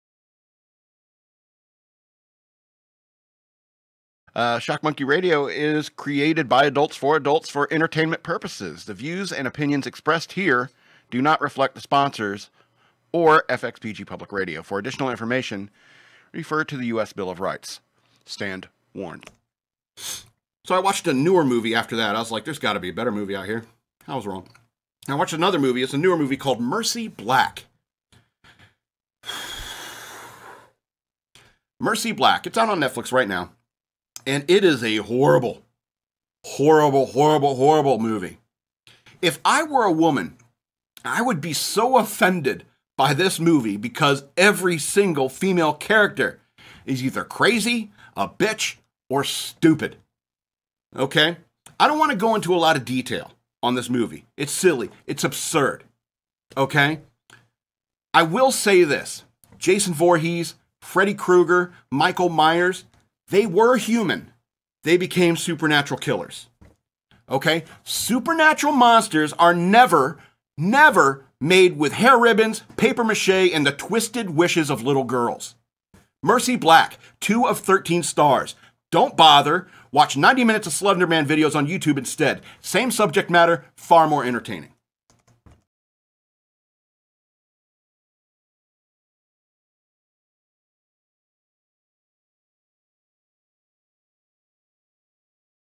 Madman Reviews the Movie "Mercy Black" SMR 07 JUL 20
The madman really hated "Mercy Black"...